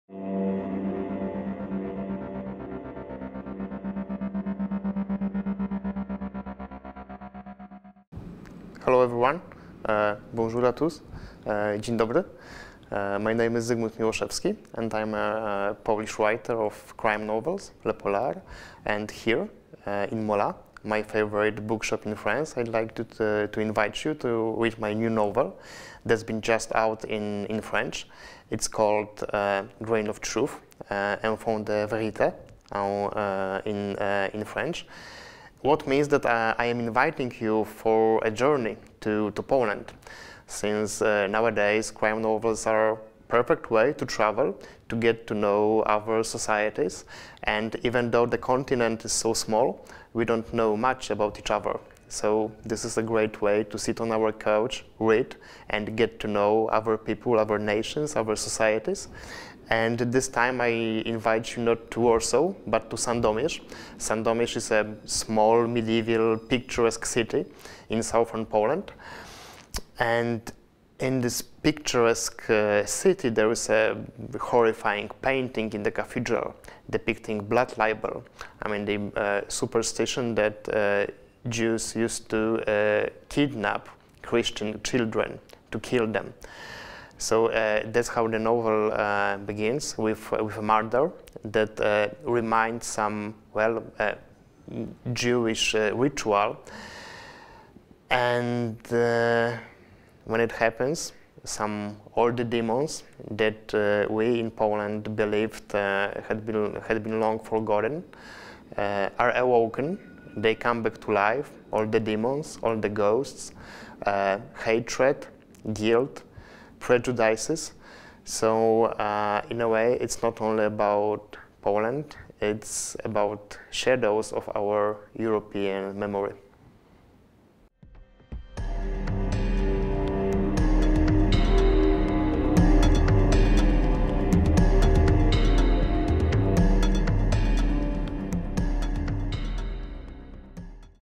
Mollat Polar # 4 - Zygmunt Miloszewski
Zygmunt Miloszewski était l'invité de notre Mollat Polar # 4. Il nous présente son ouvrage "Un fond de vérité" aux éditions Mirobole. Traduit du polonais par Kamil Barbarski.